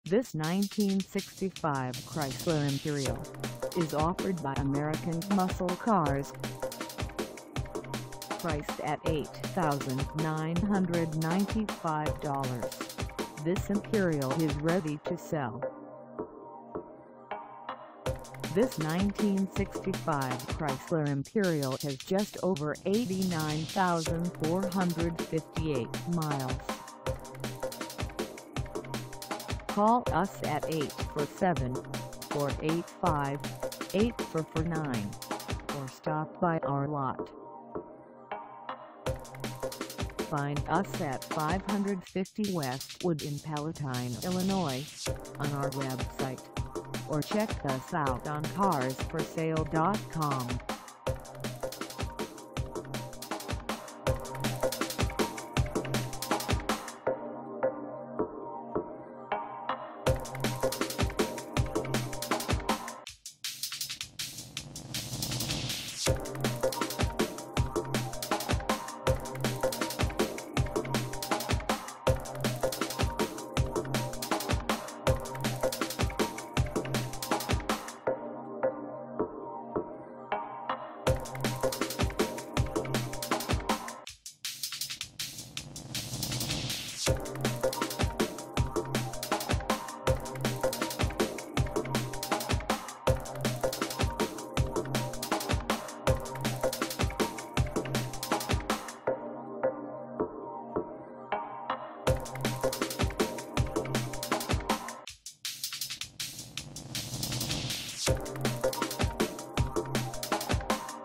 1965 Chrysler Imperial Rare - for sale in Palatine, IL 60067
550 West Wood
in Palatine, IL 60067

Come test drive this 1965 Chrysler Imperial Rare for sale in Palatine, IL.

andnbsp;Mileage: 89,4584 DoorBlack Leather InteriorNEW: Fuel tank, brakes and lines, re-built carburetor and battery.Only Flaws:Rear driver's side window doesn't go down.Rear passenger door doesn't open.Hood needs to be re-alignedWE SHIP WORLDWIDE-SHOWN BY APPT ONLY